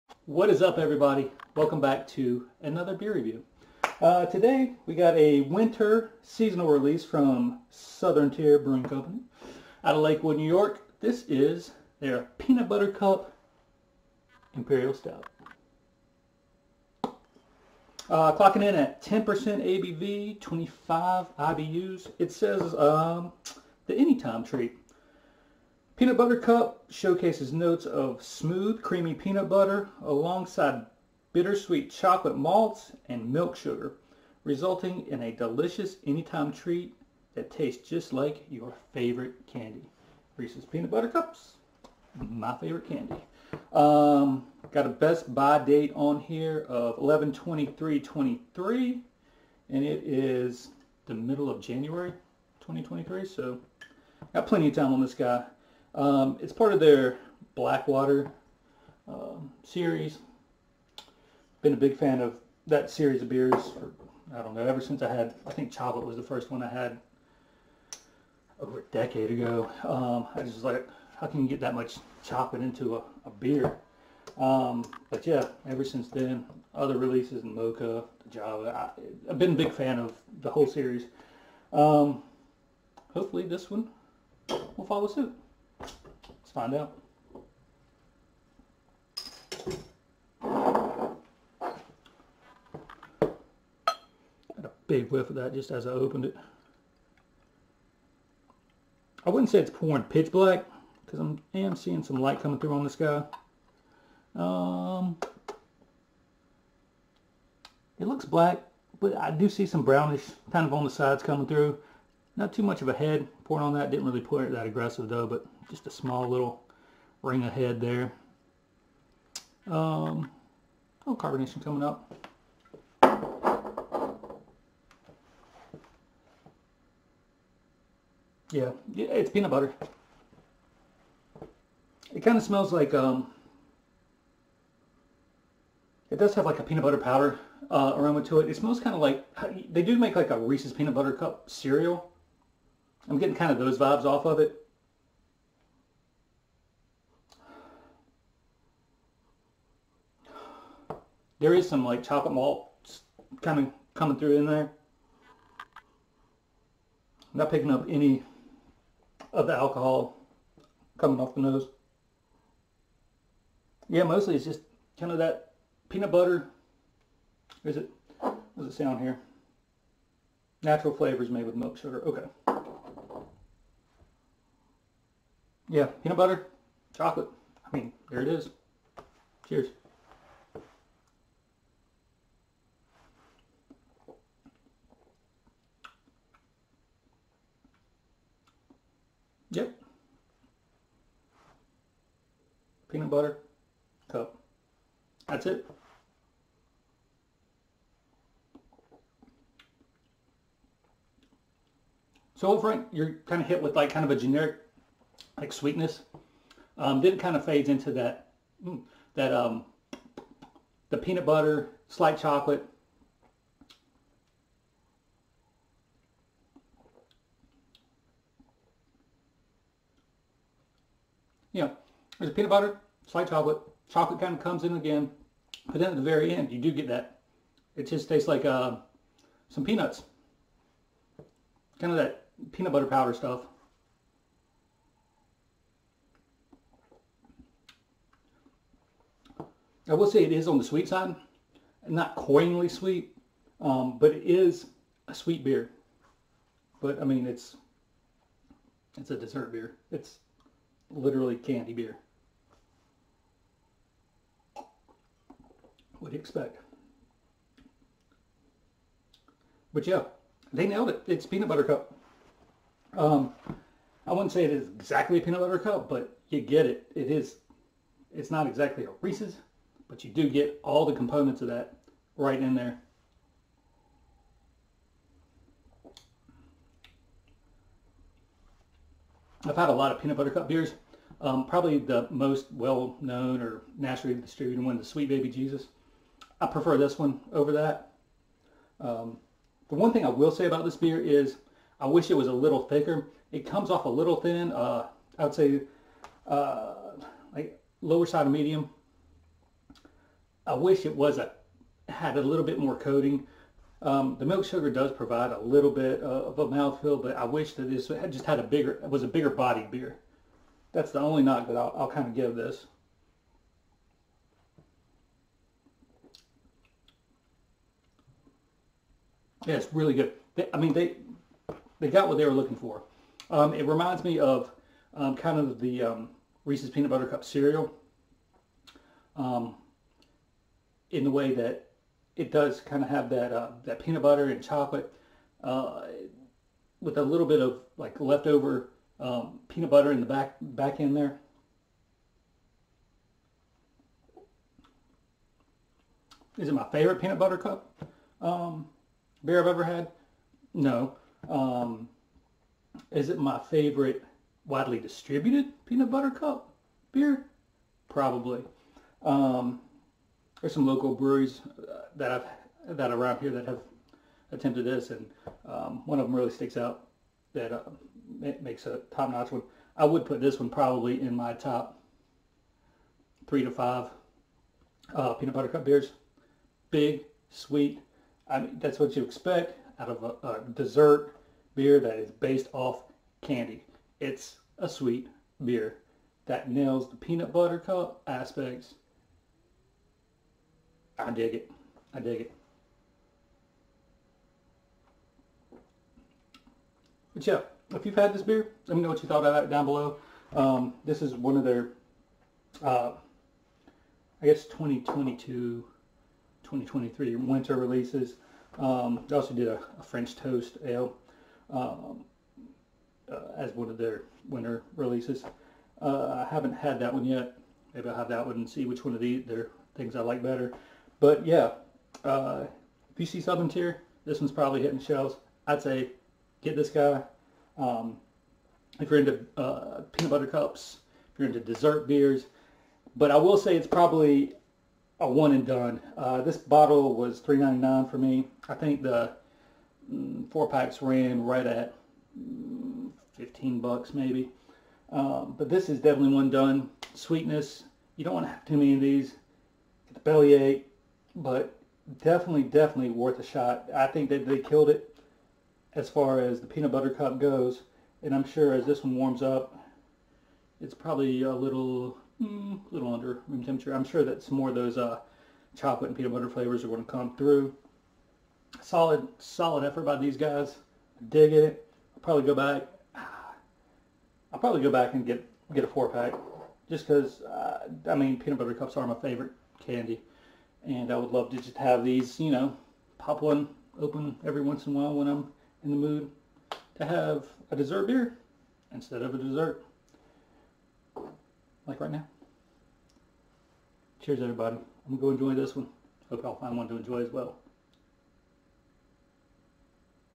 Peanut Butter Cup (Imperial Stout) | Southern Tier Brewing Co. | Beer Review
Today we try out a winter release from Southern Tier Brewing Company (Lakewood, NY) - their Peanut Butter Cup Imperial Stout.

Peanut Butter Cup showcases notes of smooth, creamy peanut butter alongside bittersweet chocolate malts and milk sugar, resulting in a delicious anytime treat that tastes just like your favorite candy.

10% ABV
25 IBU
Best By date of 11/23/2023 (reviewed mid January 2023)